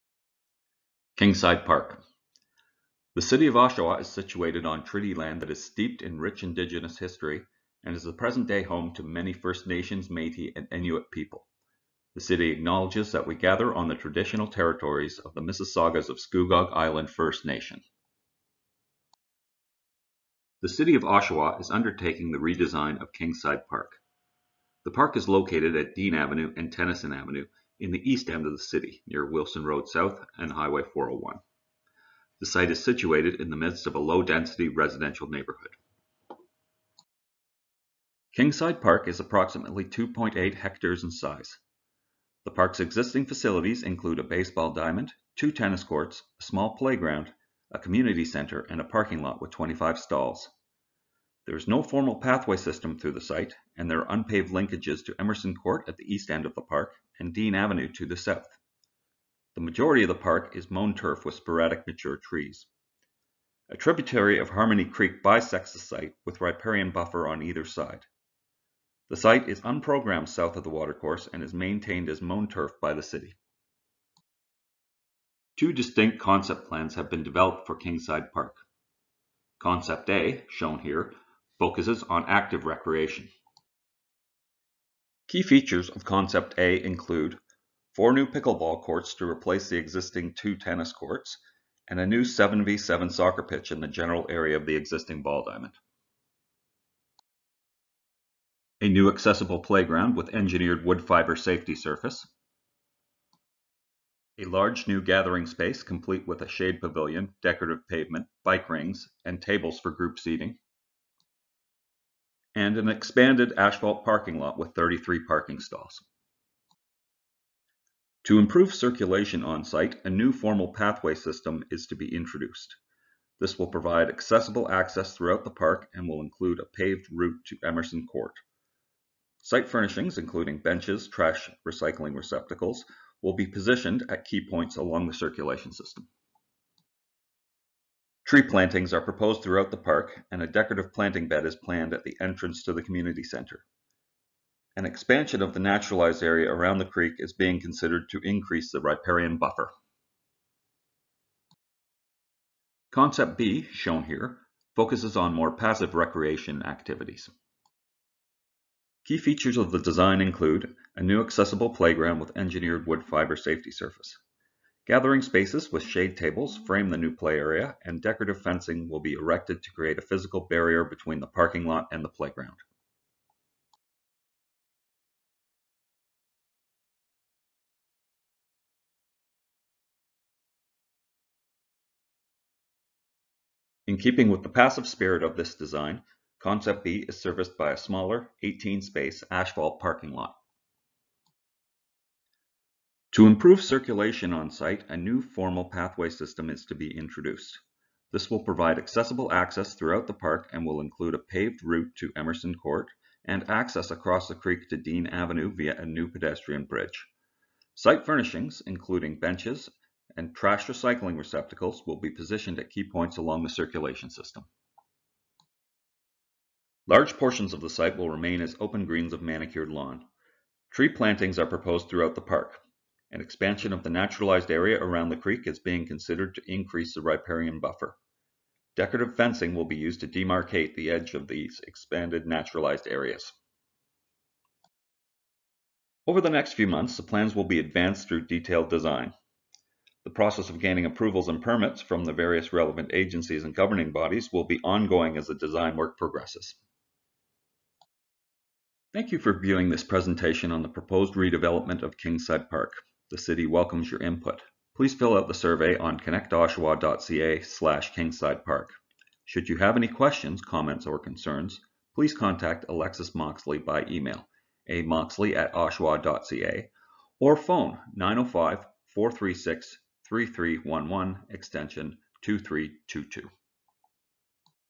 Kingside Park Redevelopment Virtual Open House Presentation
The City of Oshawa invites community members to learn more about the proposed redevelopment of Kingside Park at 537 Tennyson Avenue by viewing this virtual open house presentation.

See the conceptual plans and then complete an online feedback form on Connect Oshawa

Feedback will be received until noon on Thursday, September 9, 2021, and will be considered in the selection of the preferred conceptual plan and playground equipment.